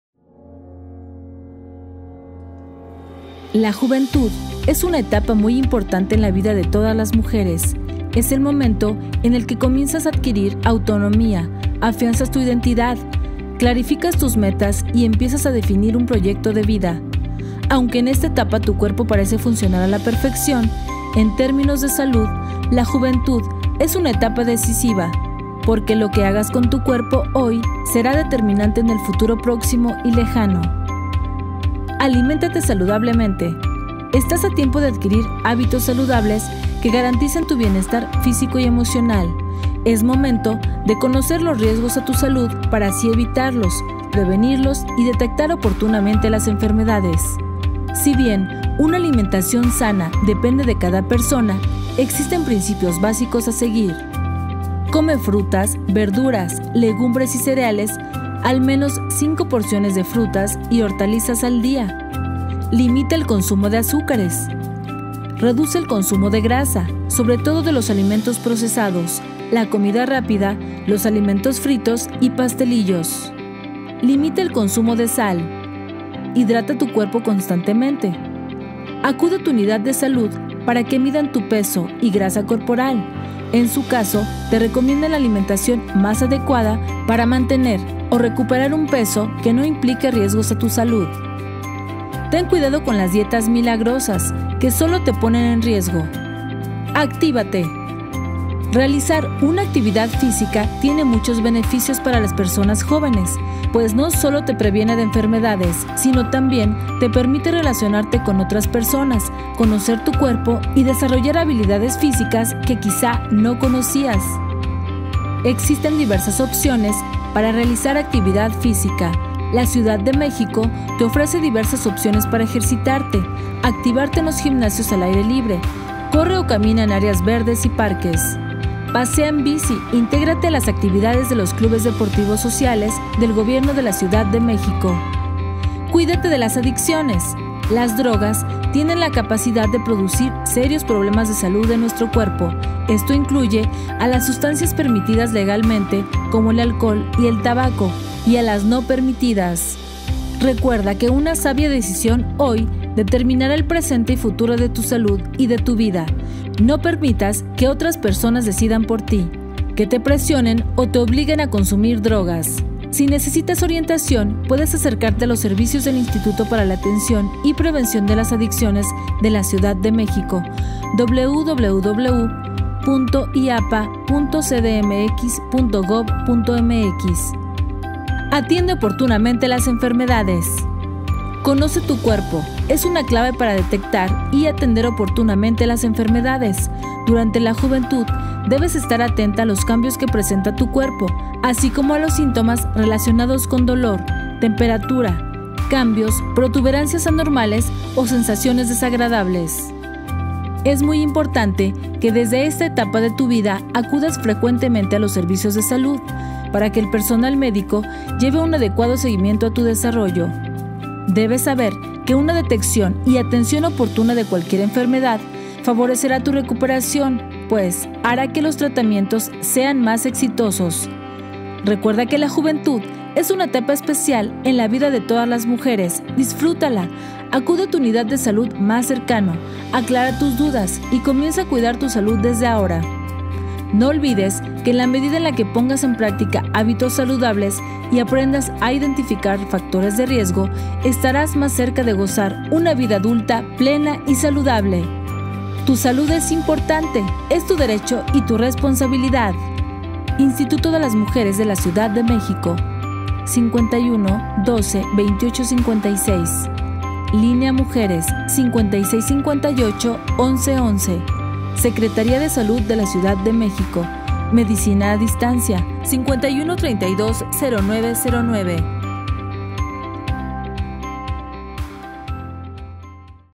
MUJERES JÓVENES PREVENCIÓN DE ENFERMEDADES
Es momento de conocer los riesgos a tu salud para así evitarlos,  prevenirlos y detectar oportunamente las enfermedades. Ejercítate y aliméntate sanamente.